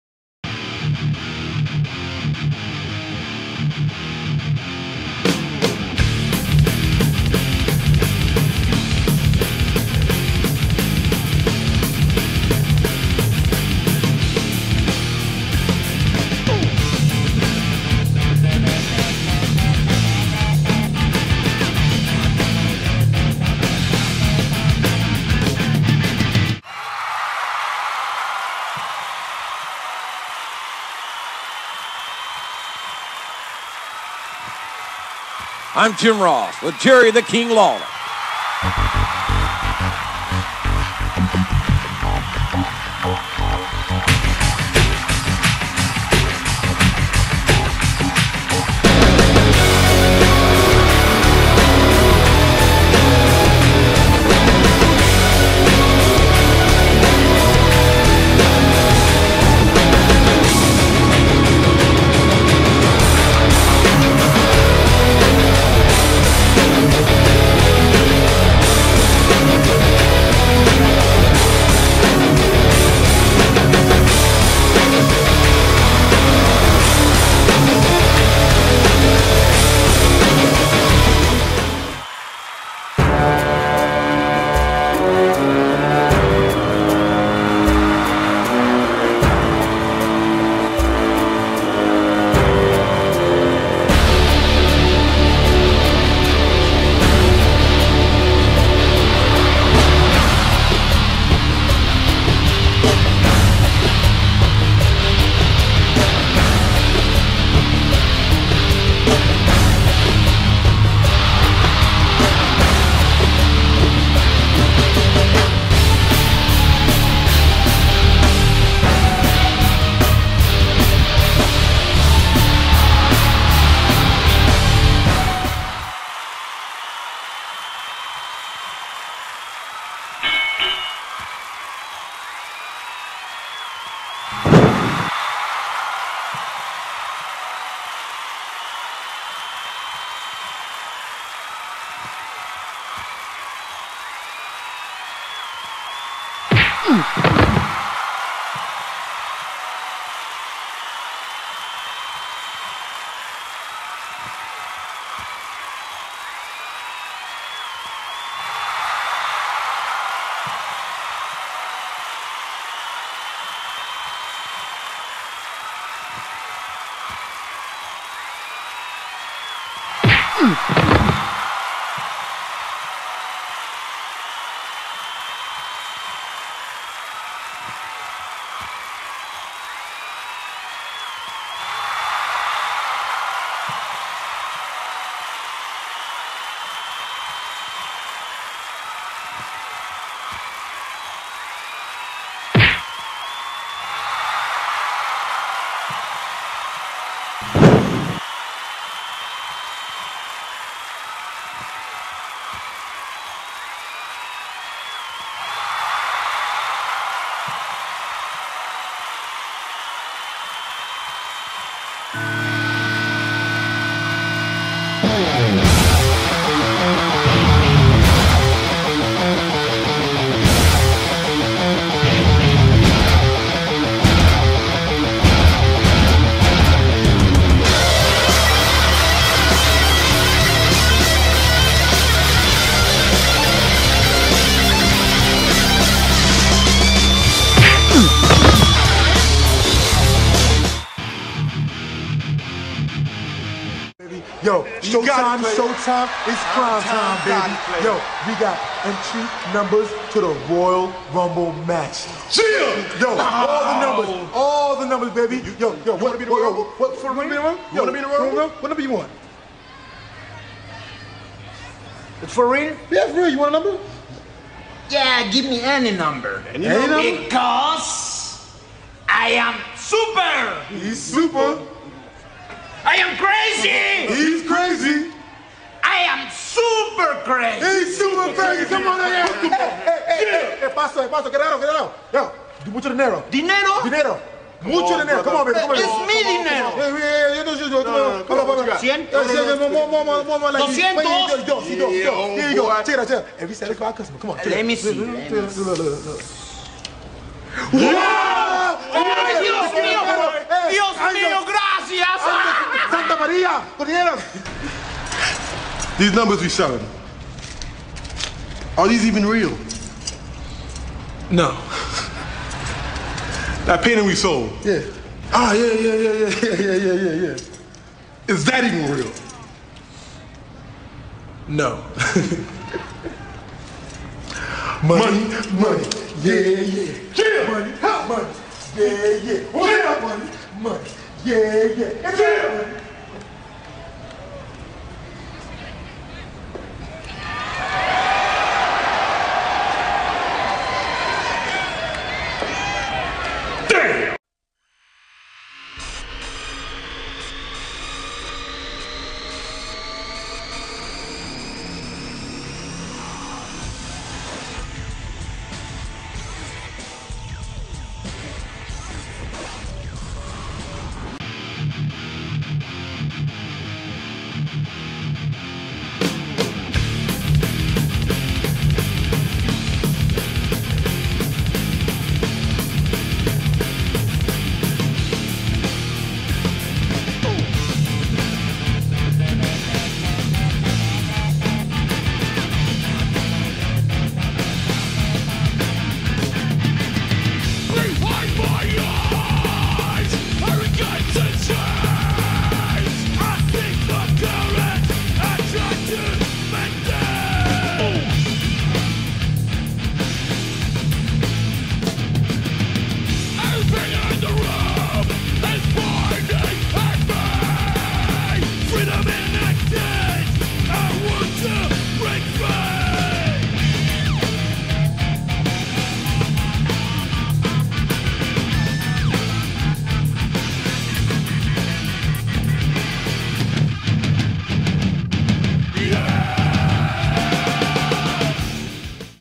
CCW Ultimate Destruction PPV (Part 1)
A stop-motion animation wrestling show with WWE and TNA action figures! Enjoy!